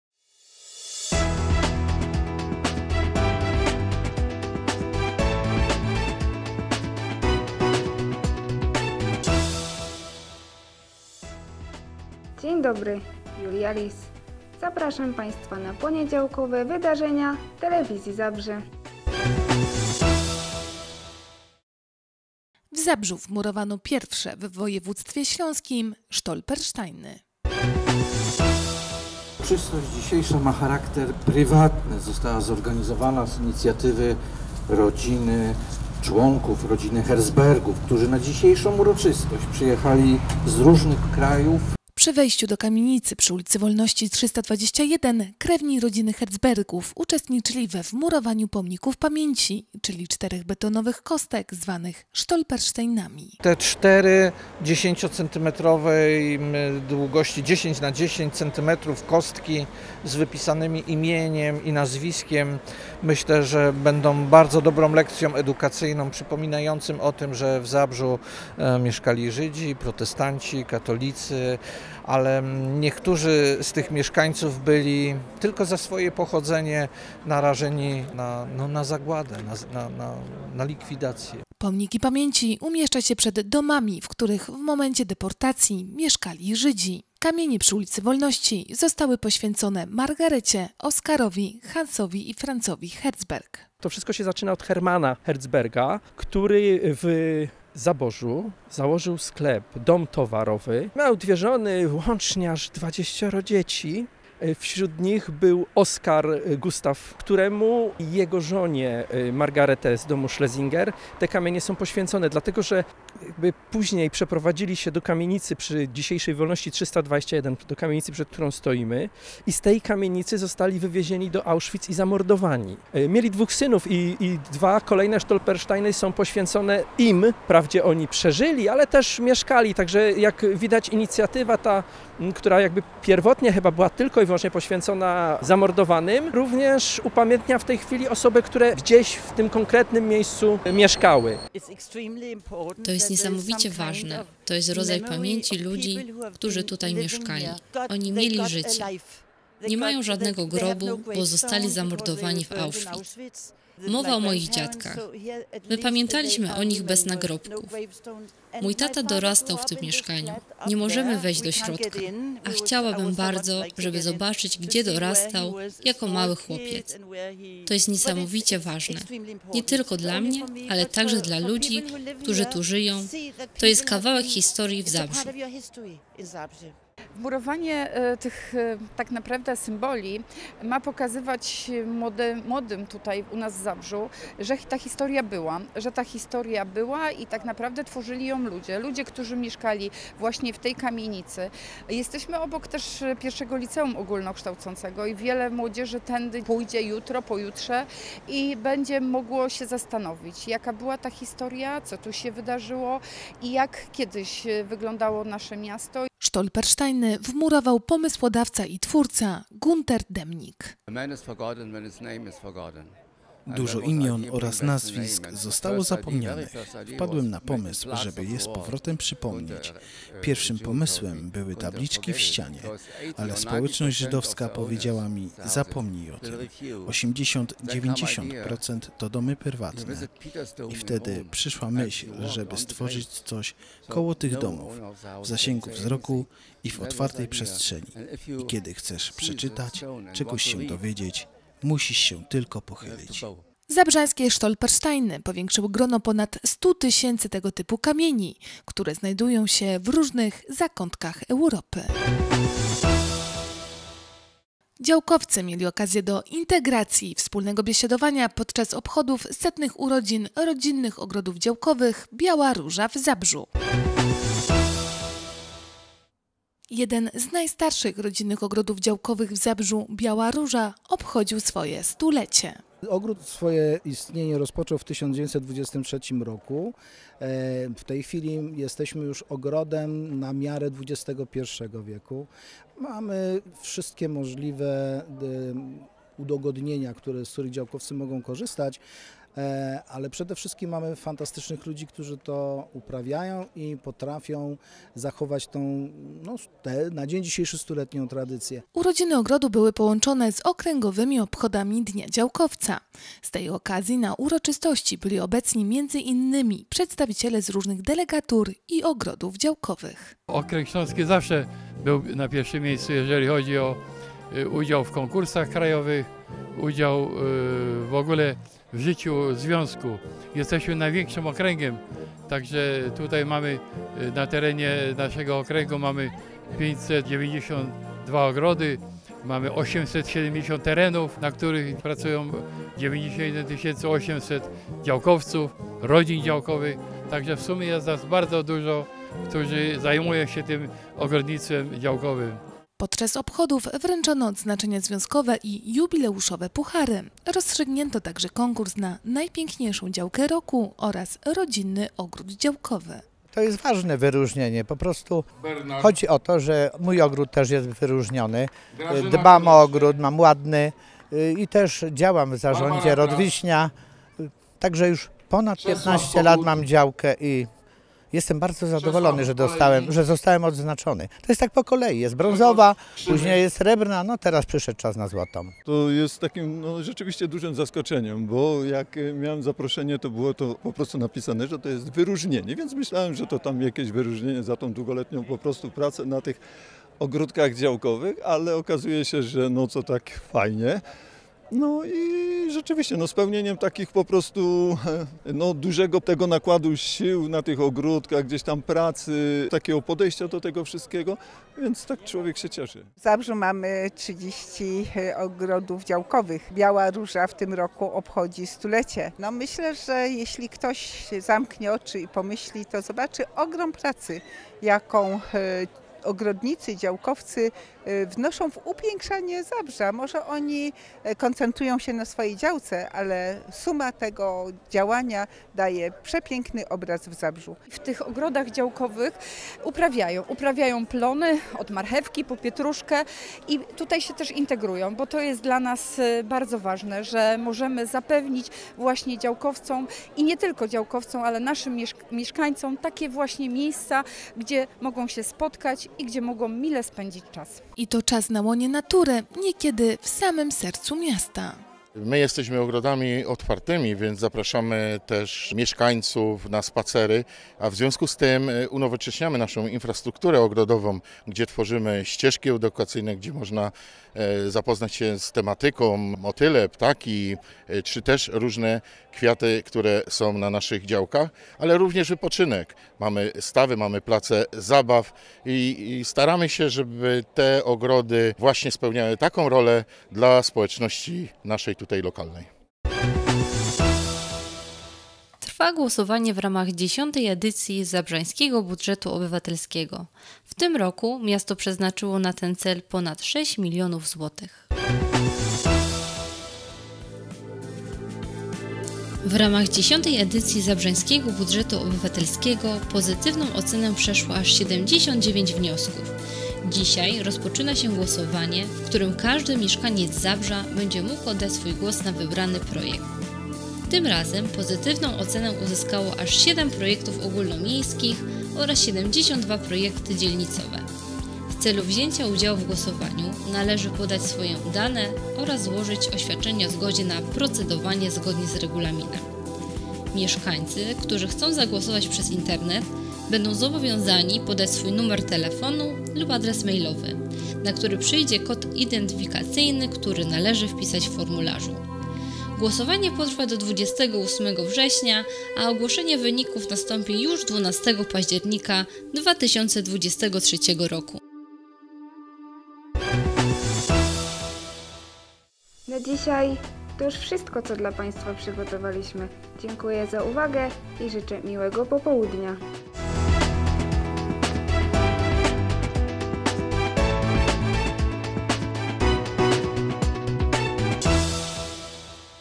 WYDARZENIA - SERWIS INFORMACYJNY (11.09.2023)
1. W Zabrzu wmurowano pierwsze w województwie śląskim stolpersteiny.

2. Działkowcy mieli okazję do integracji i wspólnego biesiadowania podczas obchodów setnych urodzin Rodzinnych Ogrodów Działkowych „Biała Róża” w Zabrzu.

3.  Trwa głosowanie w ramach X edycji Zabrzańskiego Budżetu Obywatelskiego. W tym roku miasto przeznaczyło na ten cel ponad 6 mln. zł.